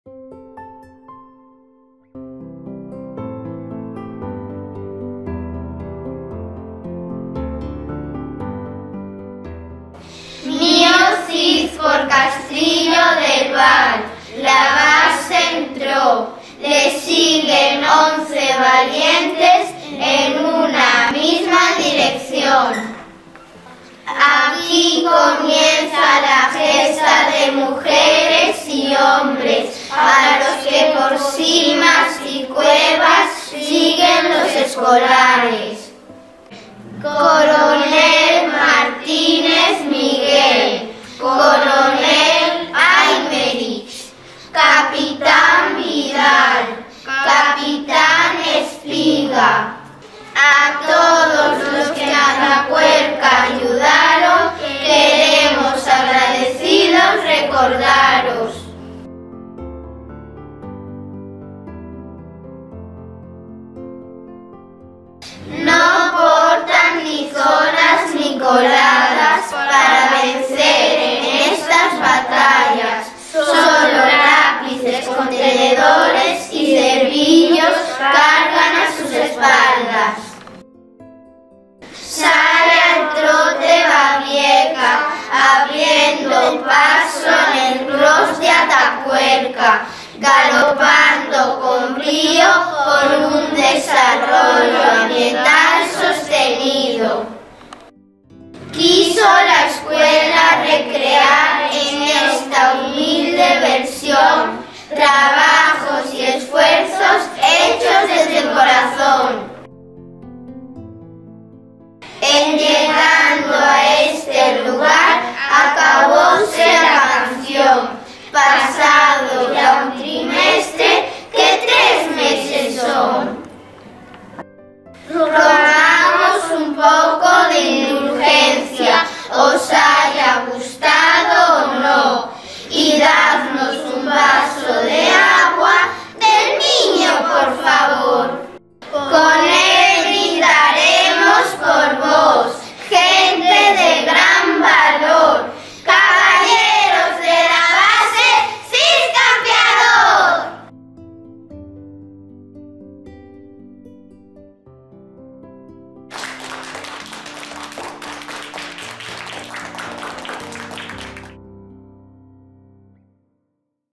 Paseando por Castrillo del Val
Vídeo realizado polo alumnado de 3º de Ed. Primaria para o seu proxecto "De base a base y leo porque me toca".